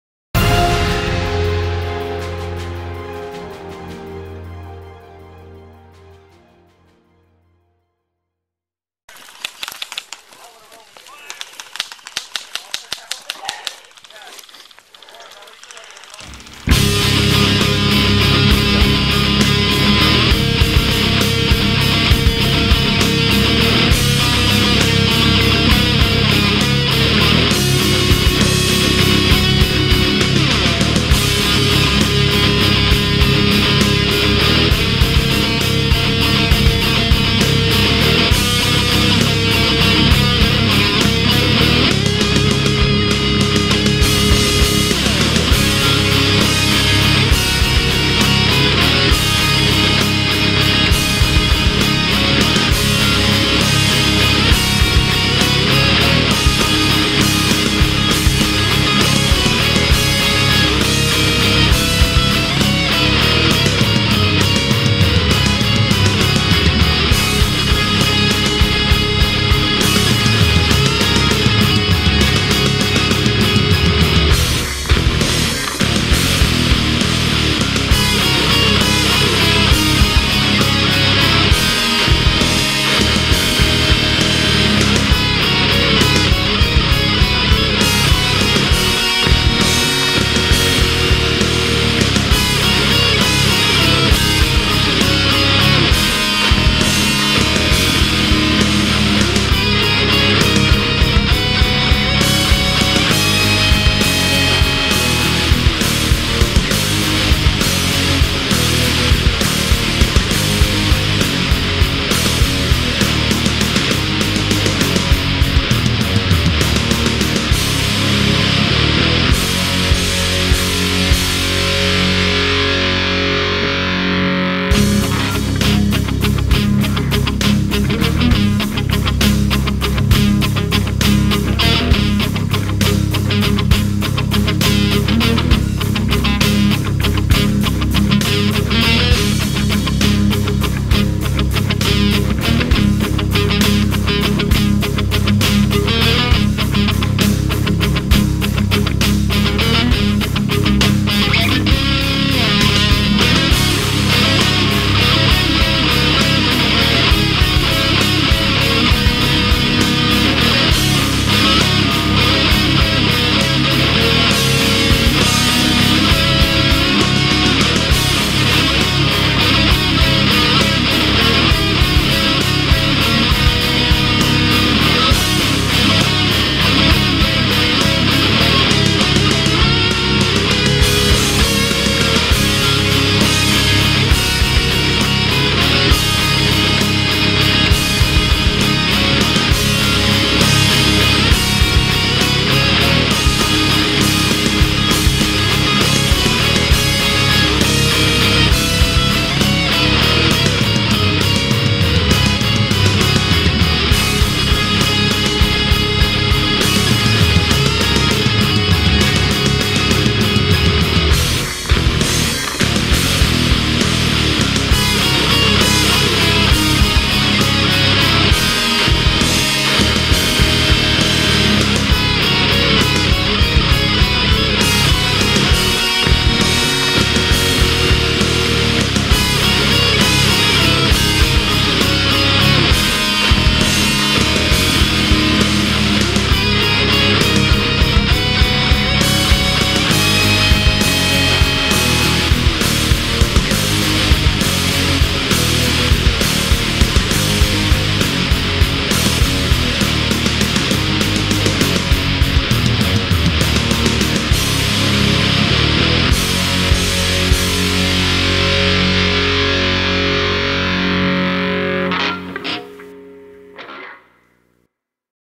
Onyx Corps Airsoft Airsplat Army Uprise Tower Def
Third of 3 videos from the Airsplat Army Operation:Uprise event on 2-22-14 at the War Room Field in madera ca.